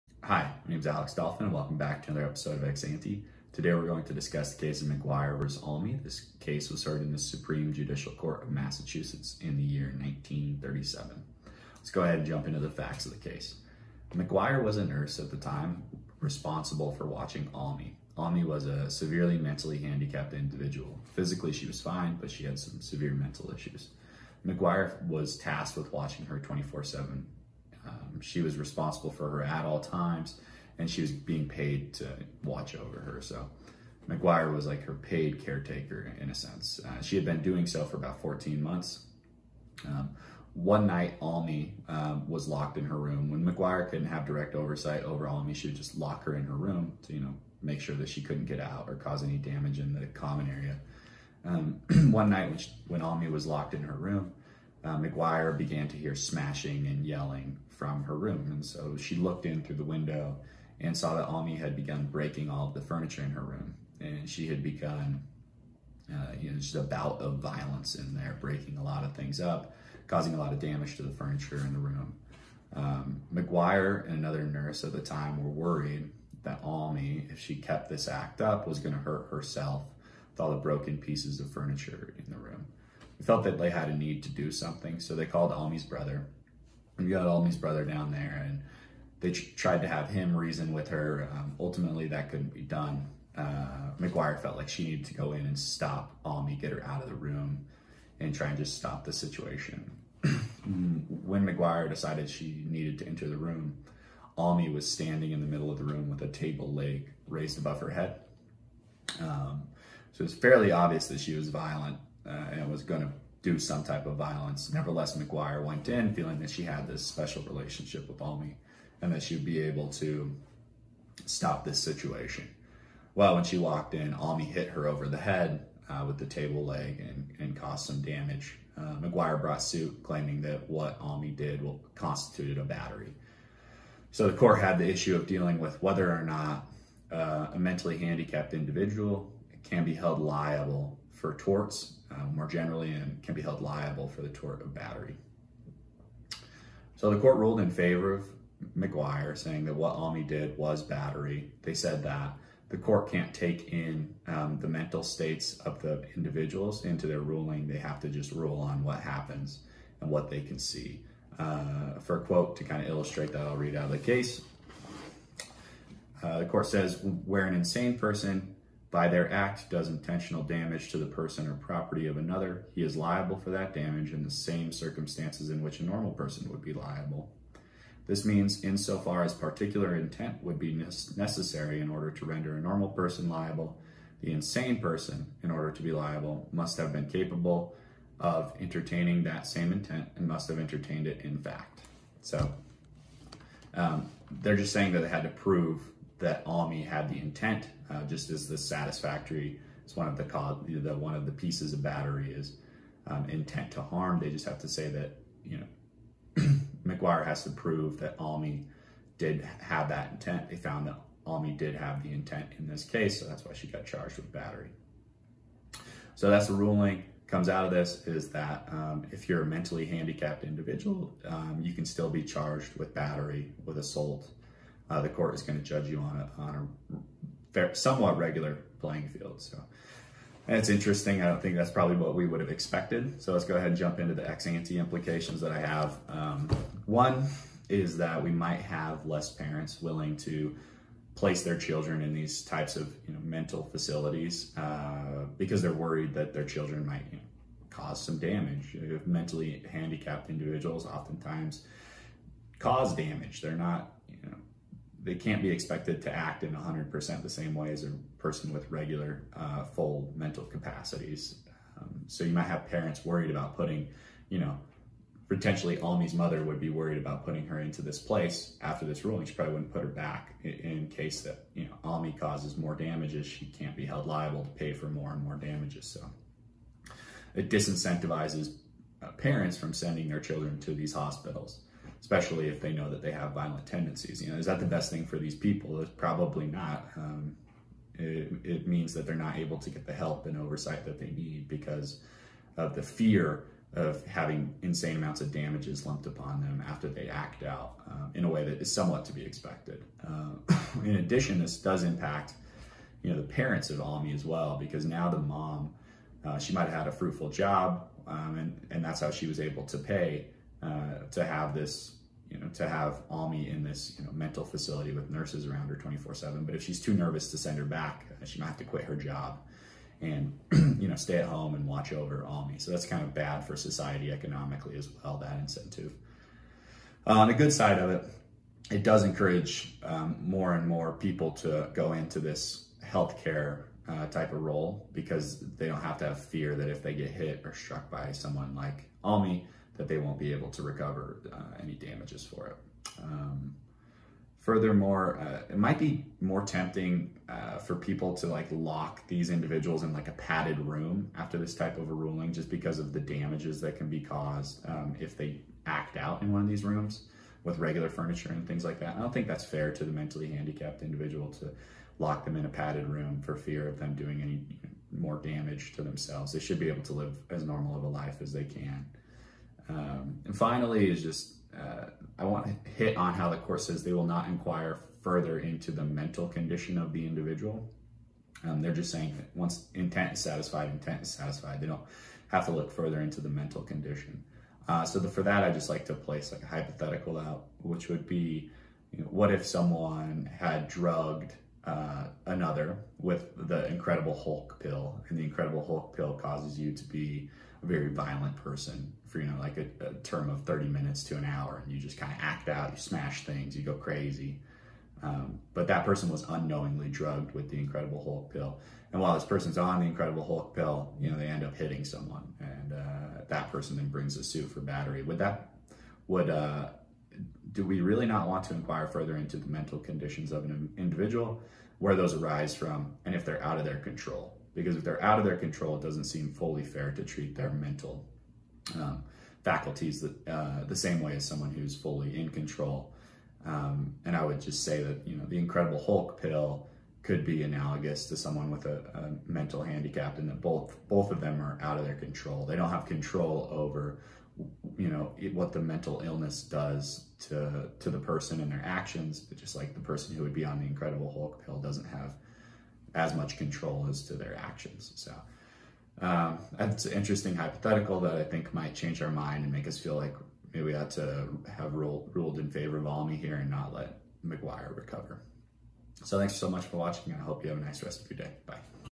McGuire v Almy | Battery
Case summary along with some implications of the precedent. This case deals addresses whether mentally handicapped individuals can be held liable for battery.